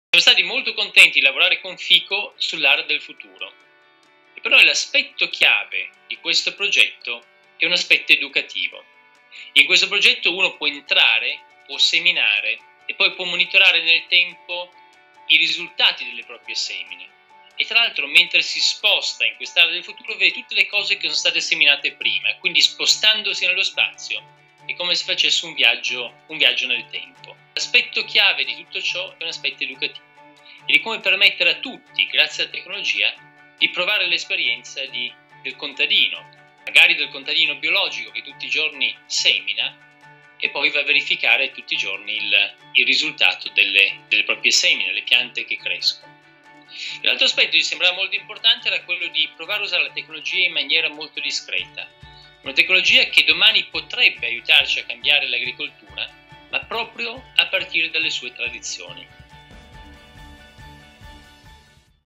FICO Eataly World dagli occhi dei protagonisti - Carlo Ratti di Carlo Ratti Associati
L'Area del Futuro che Carlo Ratti (Carlo Ratti Associati) sta progettando per FICO Eataly World, sarà un padiglione dell'agricoltura collaborativa. Un grande giardino idroponico circolare, arricchito di contenuti grazie alla realtà aumentata, dove i visitatori potranno scegliere e piantare i propri semi e vivere e studiare l'esperienza della crescita e lo sviluppo della pianta nel tempo, anche nel post FICO, da casa, tramite una app.